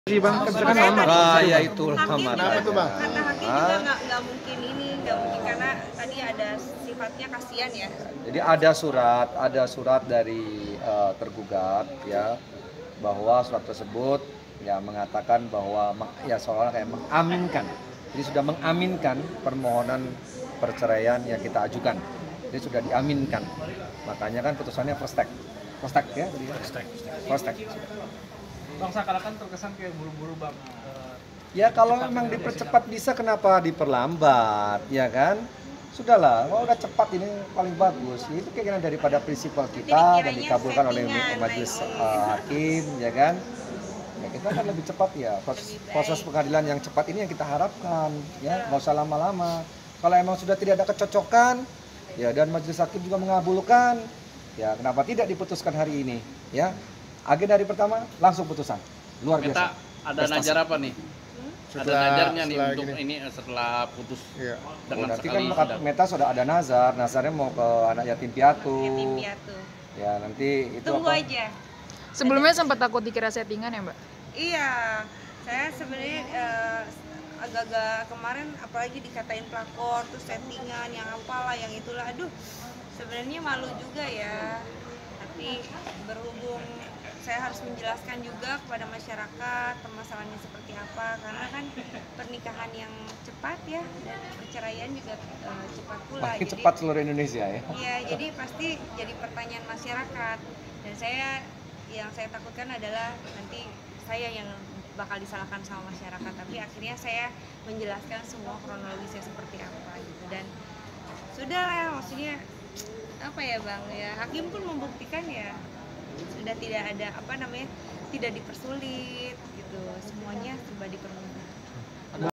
Ini Alasan Ratu Meta Sidang Pertama Langsung Dapat Putusan Cerai
Kabar mengejutkan datang dari rumah tangga penyanyi dangdut Ratu Meta. Meta yang baru menikah tiga bulan dengan Eddy Faisal bakal menggugat cerai sang suami.

Gugatan tersebut dikabulkan oleh hakim pada sidang perdananya hari ini Rabu (12/4/2019). Selengkapnya dalam video ini.